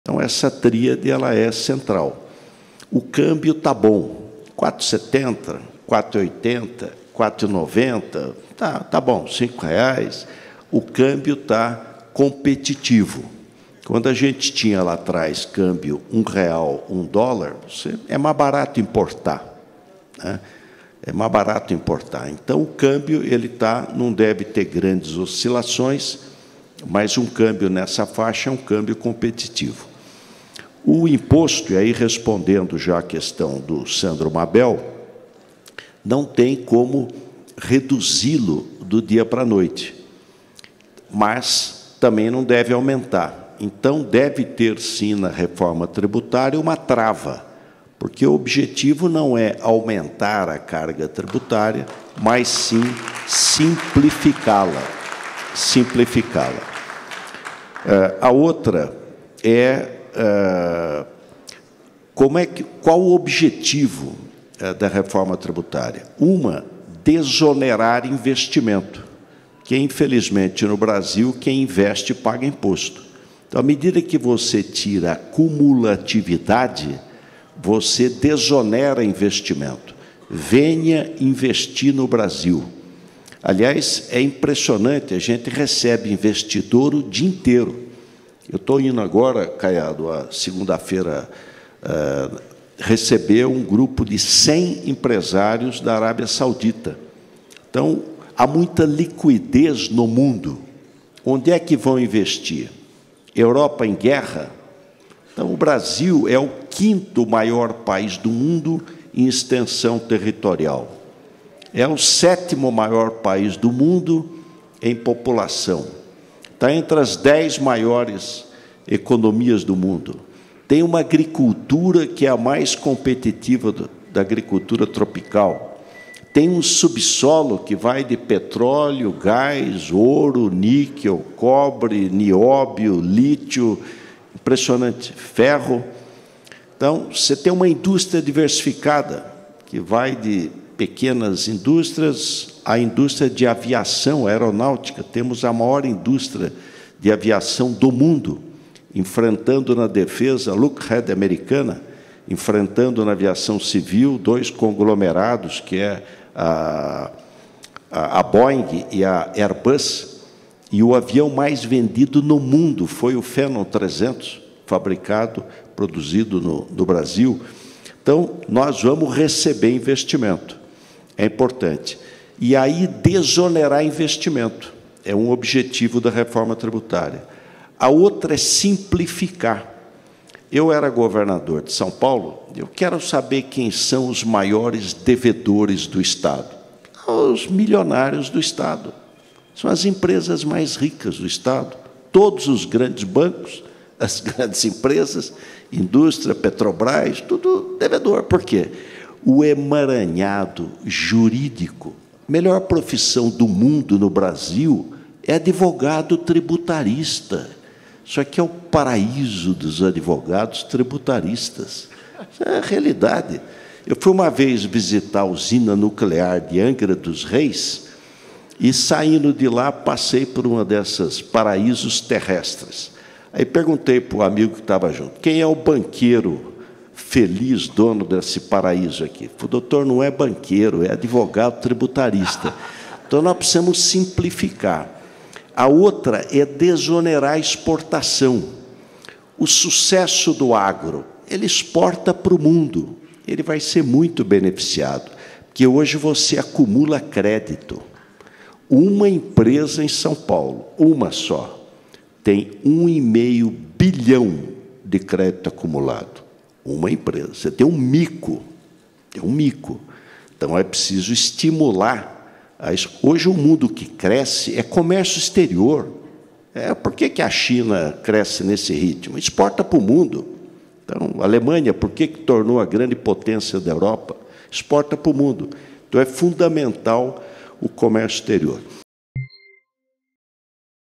‘Objetivo da reforma é simplificar carga tributária’, diz Alckmin
O vice-presidente Geraldo Alckmin (PSB) disse nesta sexta-feira (28/7) que o objetivo da reforma não é aumentar a carga tributária, e sim simplificá-la.